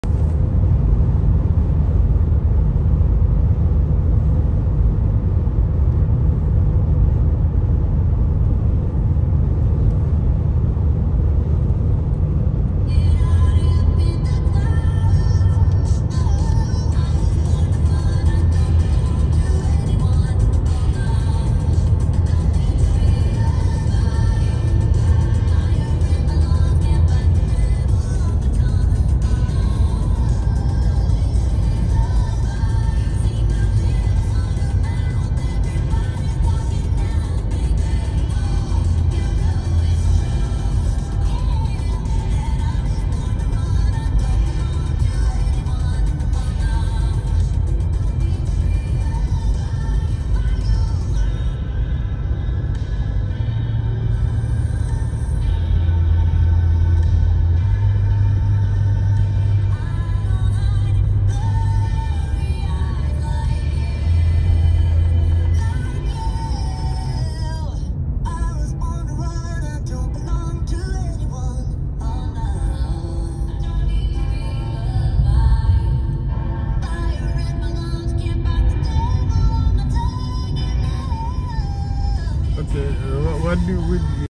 Melbourne Airport highway 2020
Melbourne Airport highway during the 2020 lockdown